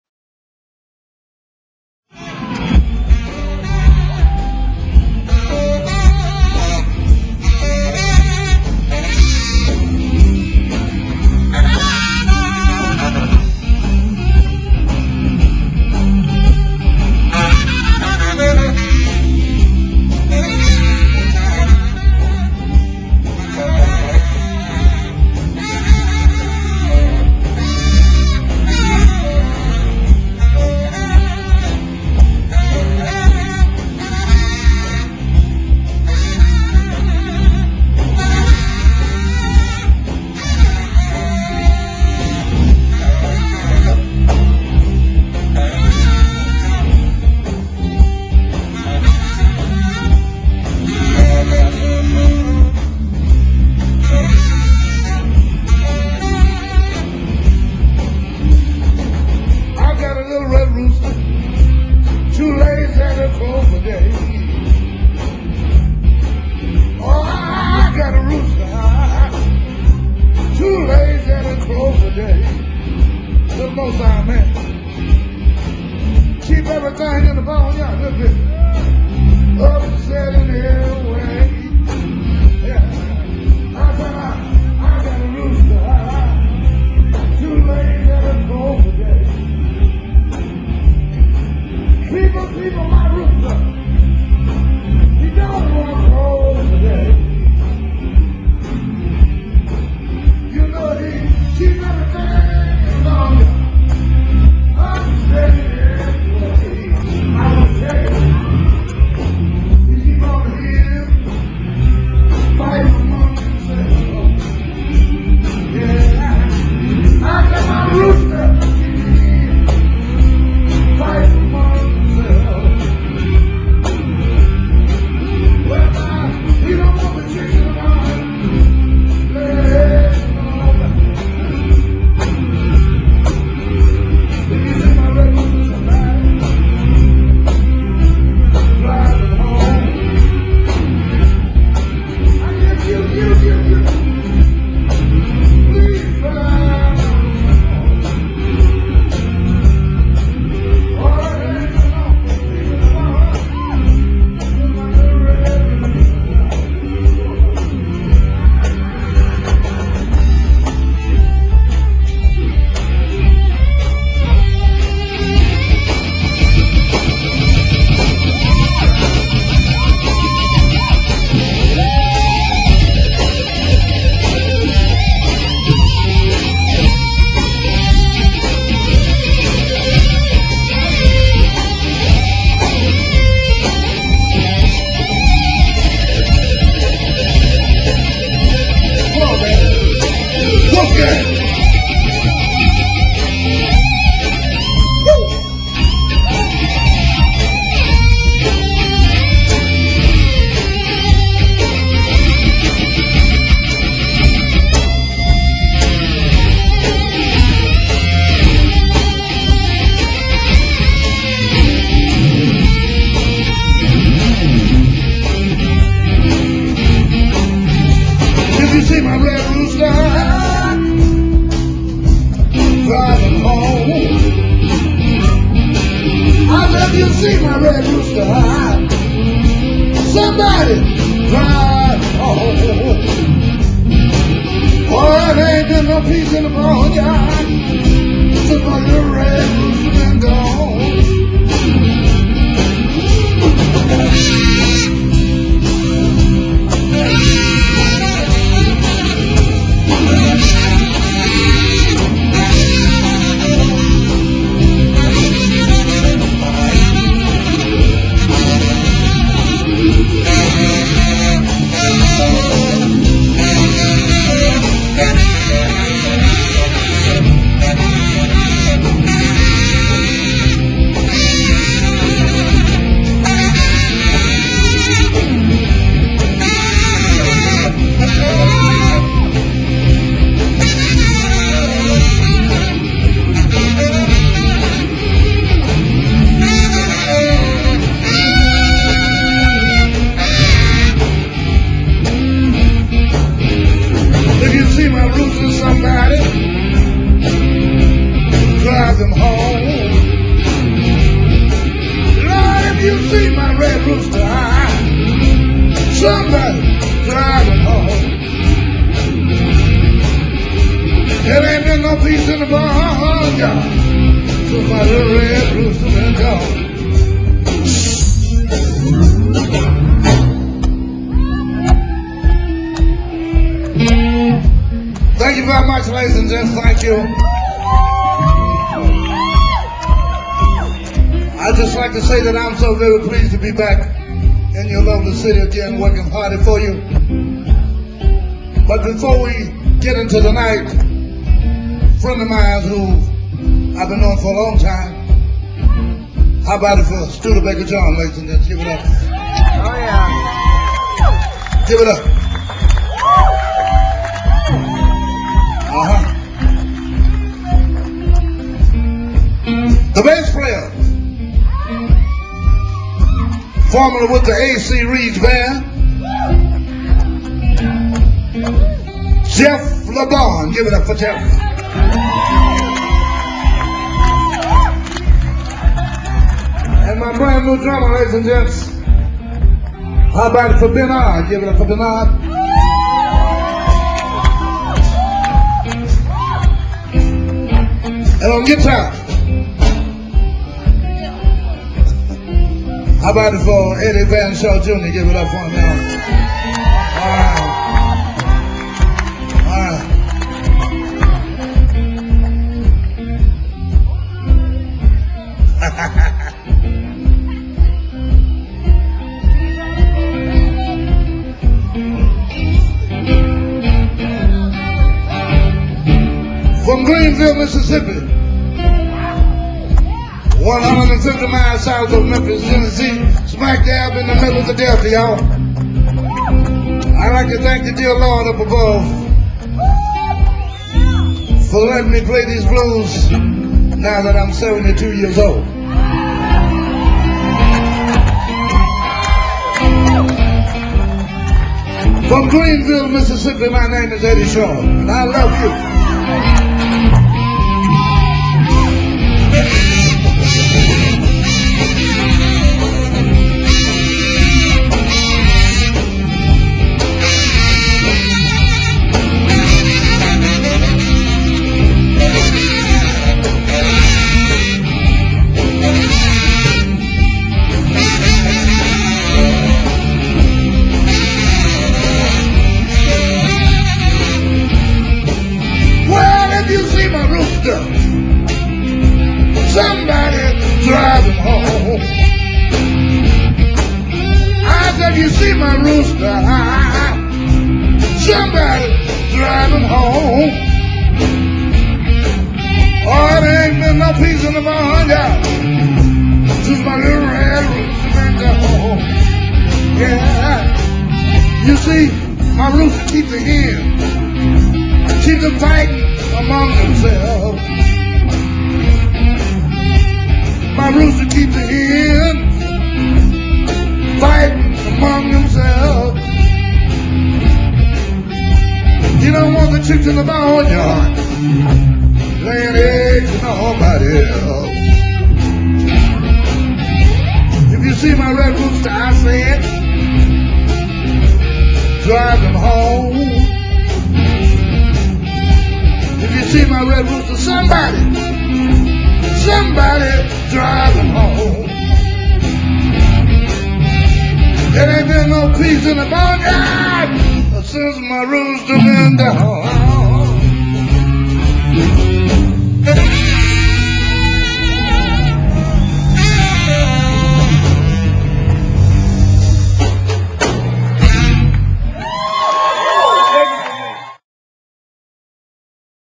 EDDIE VAAN SHAW and EDDIE SHAW(2010)winter bluesfest.
(2010)10th.annual  fargo  winter blues festival,,,north dakota